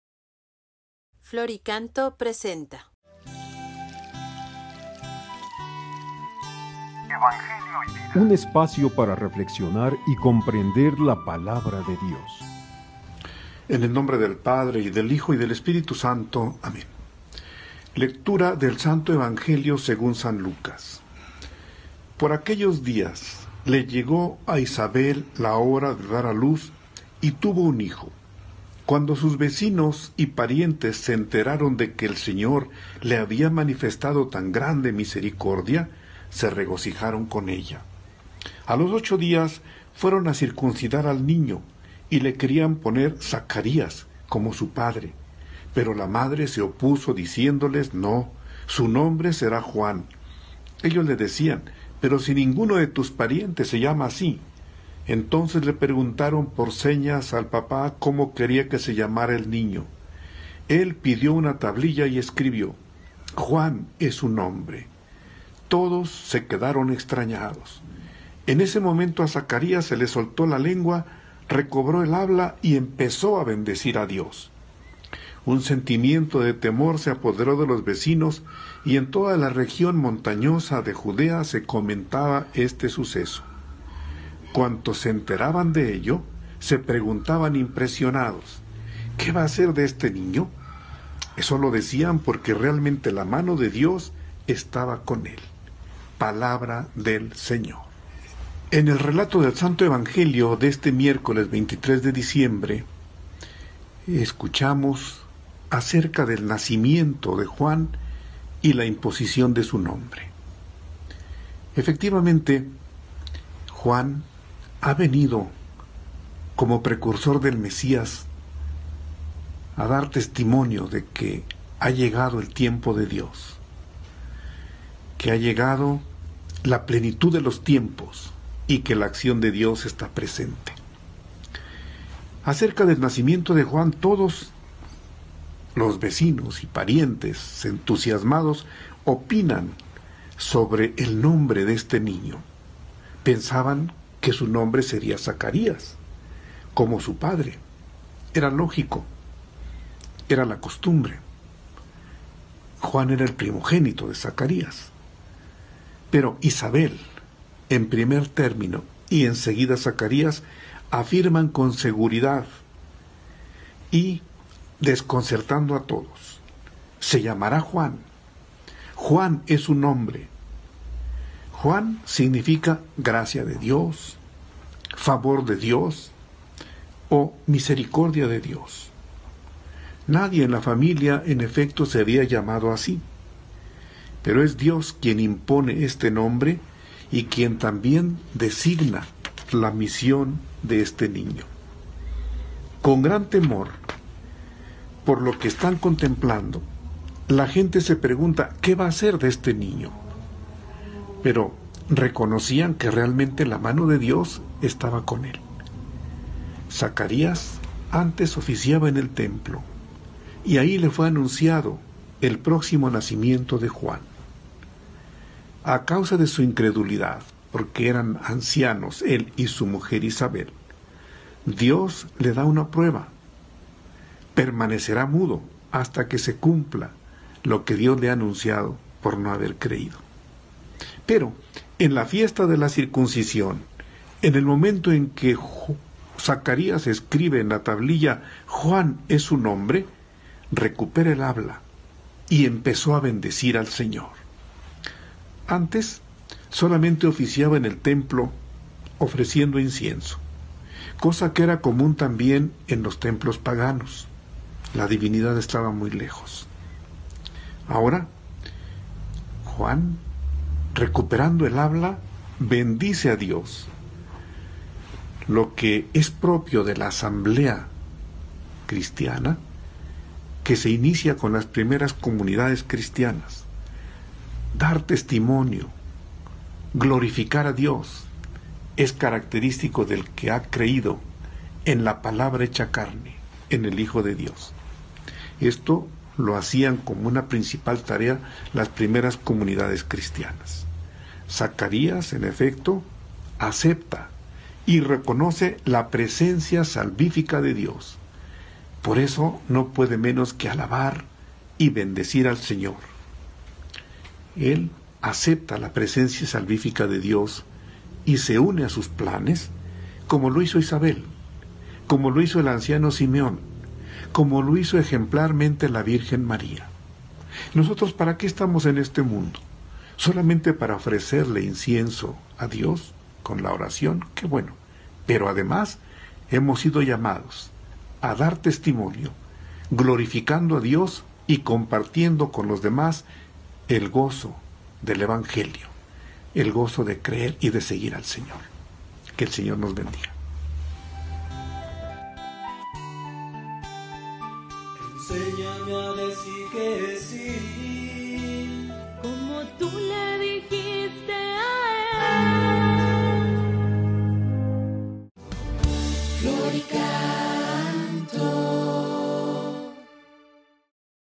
Evangelio y Vida. 23 de diciembre del 2020
S. E. Mons. Victor René Rodríguez Gómez, Obispo de la Diócesis de Valle de Chalco, nos comparte su reflexión del Evangelio de cada día y nos invita a llevarlo a la vida cotidiana.